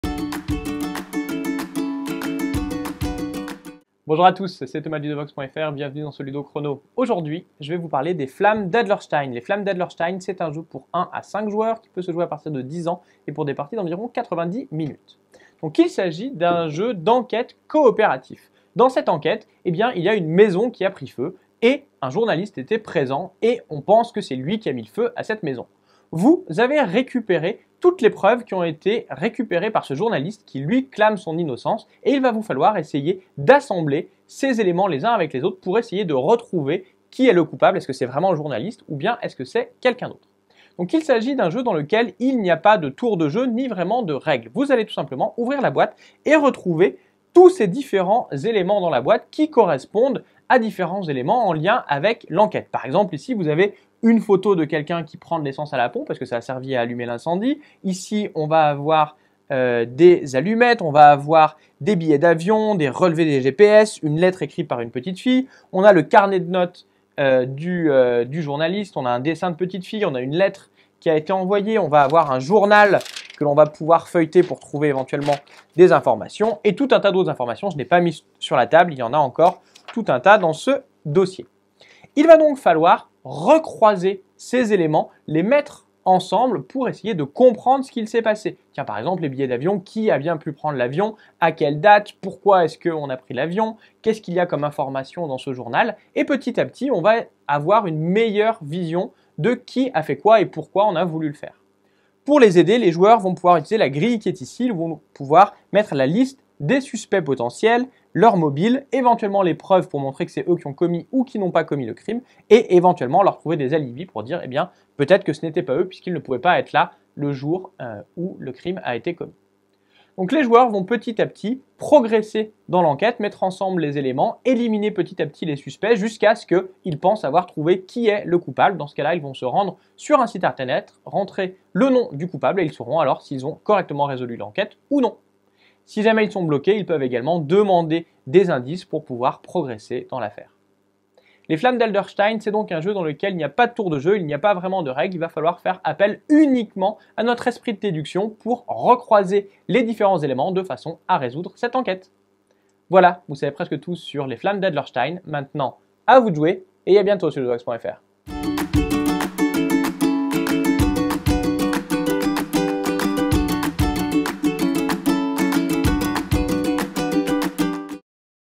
Ludochrono - Les flammes d’Adlerstein
Voici la présentation du jeu "Les flammes d’Adlerstein" avec une vue d'ensemble du fonctionnement et du matériel, le tout en 5 minutes.